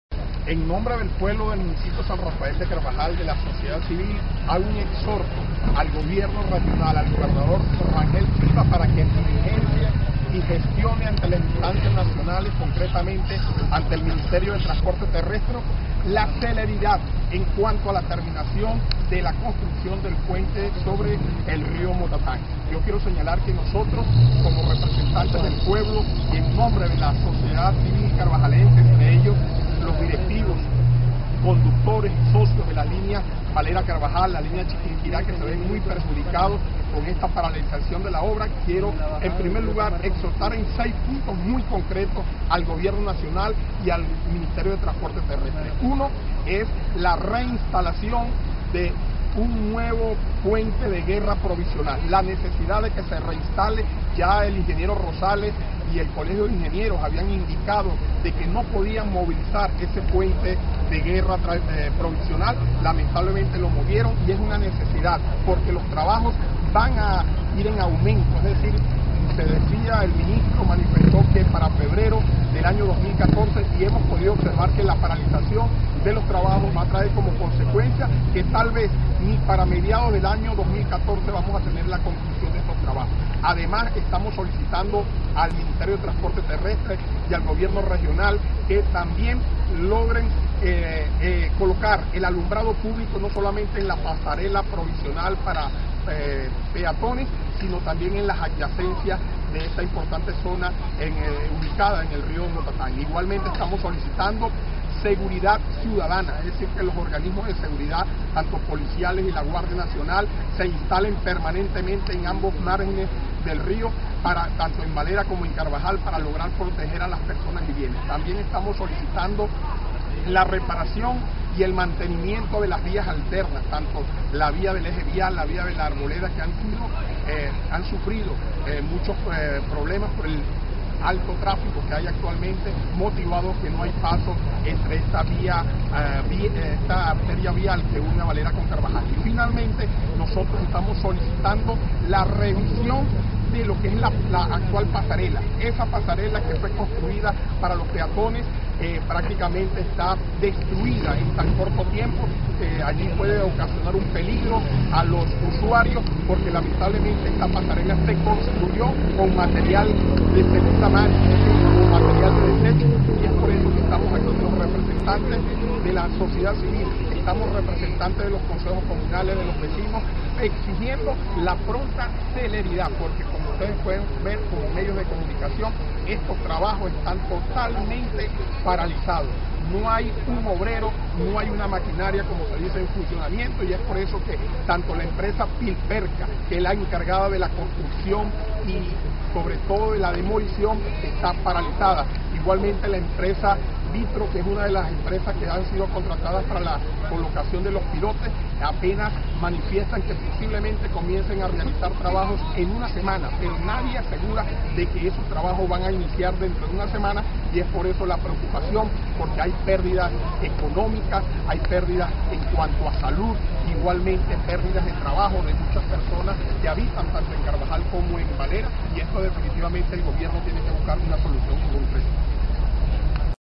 WOLFGANG PEÑA ARAUJO
Entrevista con el Alcalde de Carvajal, sobre la Construcción del Puente Sobre El Rio-Moatatn